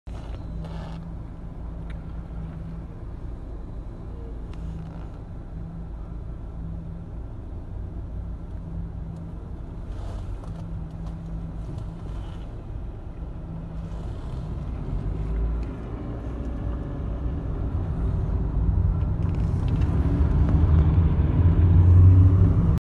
Пожар в магазине "Сотый" на площади Ленина
Видео предоставлено очевидцем Дмитрием Афанасьевым